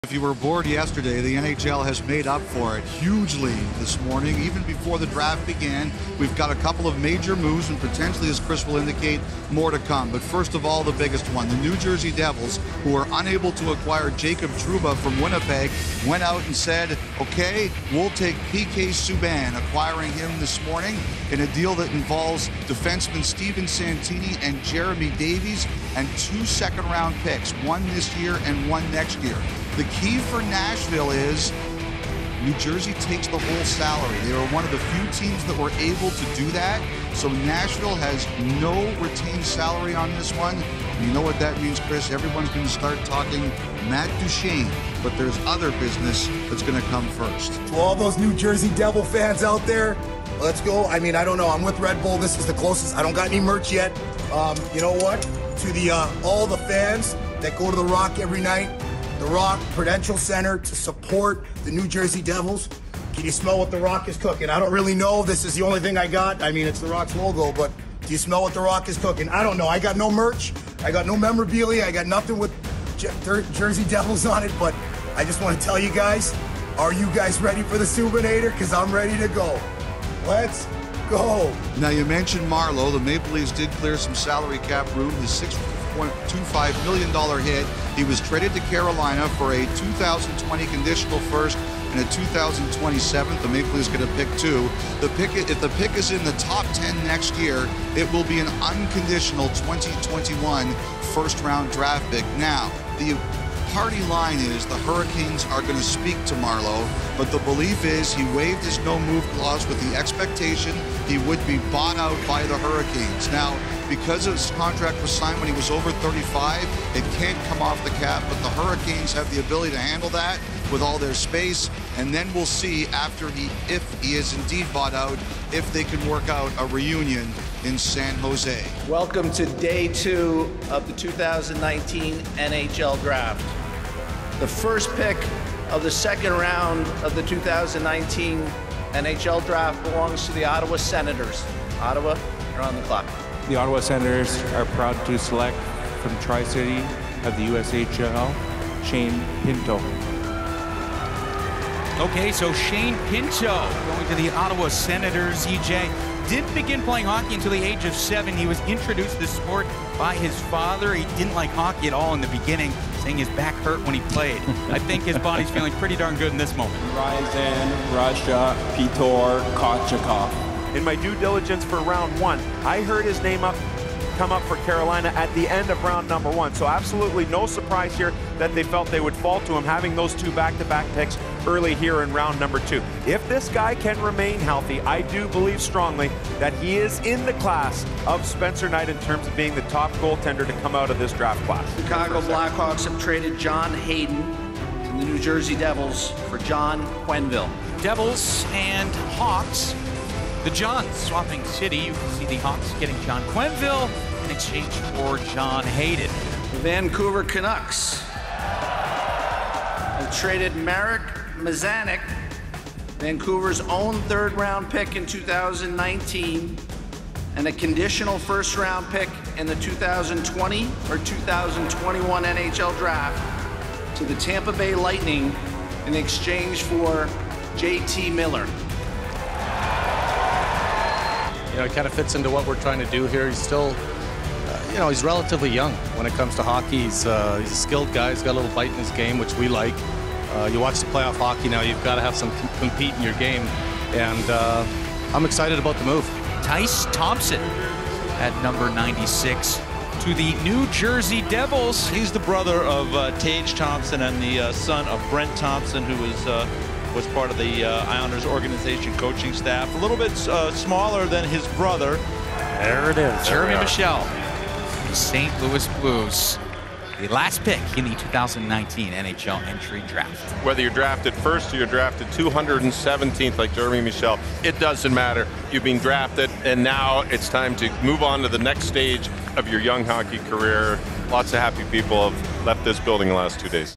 Best of the 2019 NHL Draft: Day 2
The 2019 NHL Draft continues in Vancouver for Day 2. P.K. Subban gets traded to the Devils in a blockbuster swap, teams continue to wheel and deal, and 186 players come one step closer to realizing their NHL dreams!

Feeling social?